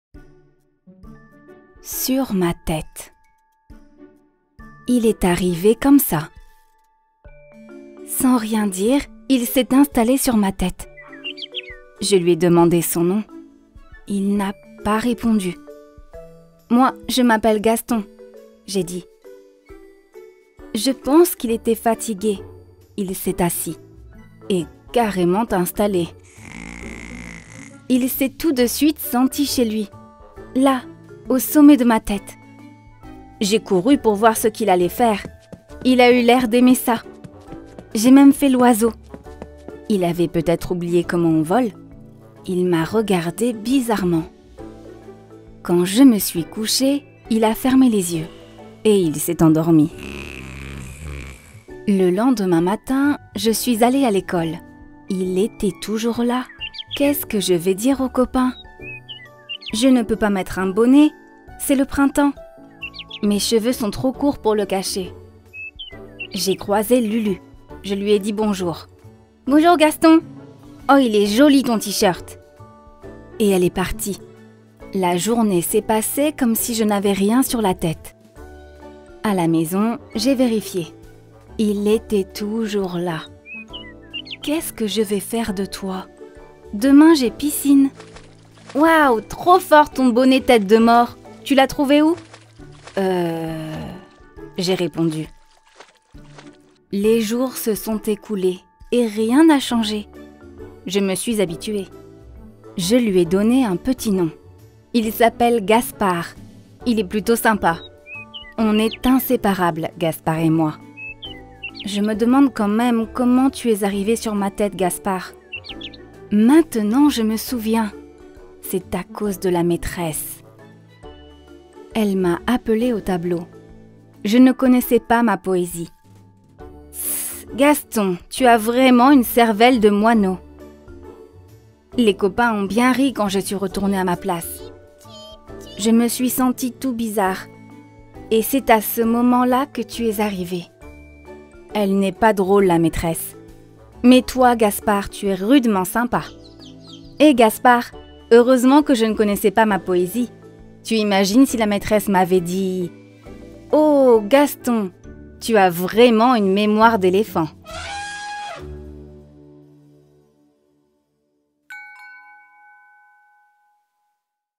Sur ma tête - Emile Jadoul
Un petit oiseau est arrivé un jour sur ma tête et y est resté. Mais je me demande comment il est arrivé là?